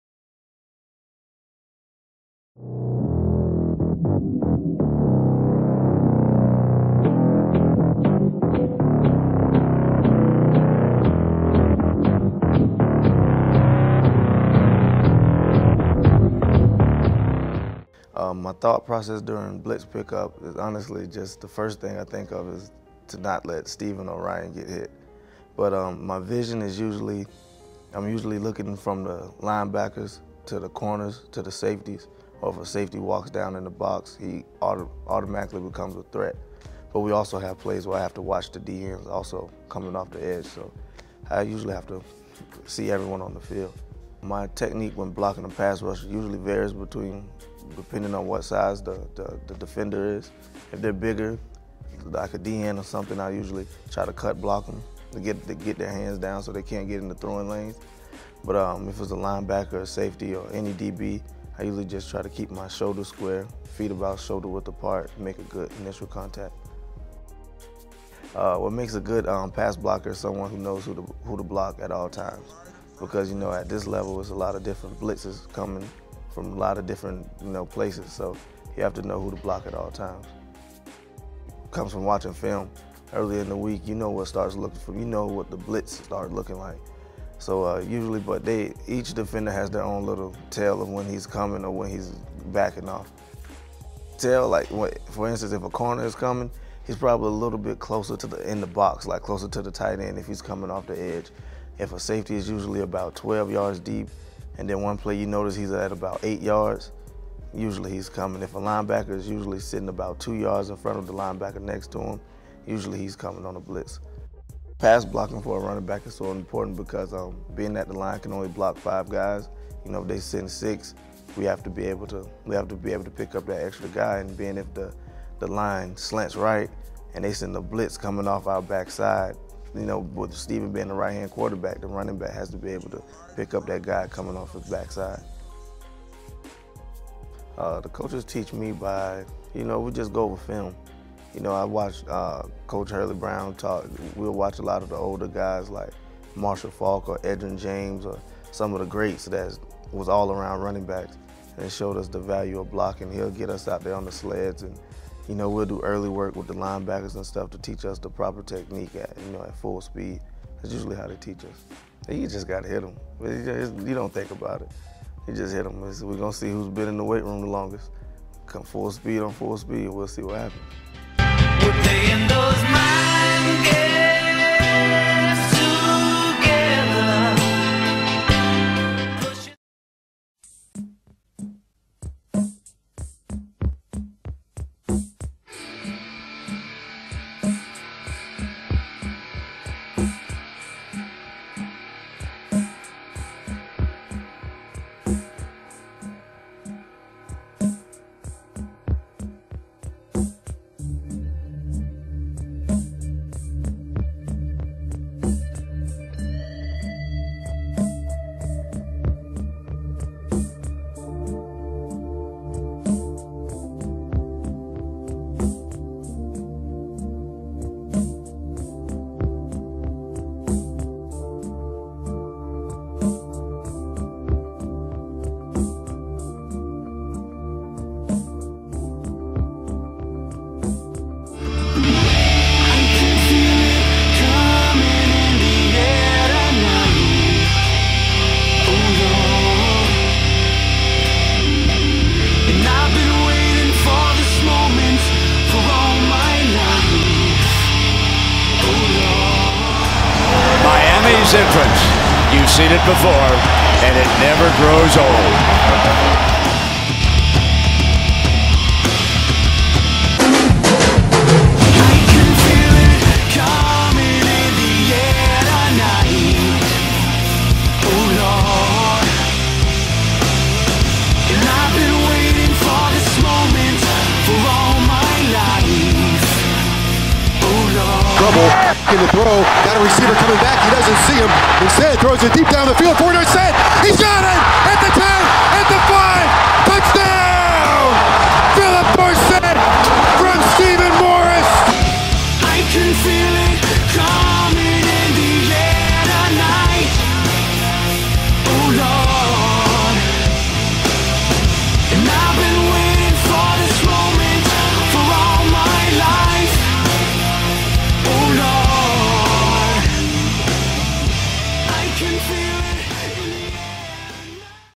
Mind Games (E31): "Blitz Pick Up"
What does Dallas Crawford think about when he's picking up the blitz? He tells you in the latest MindGames.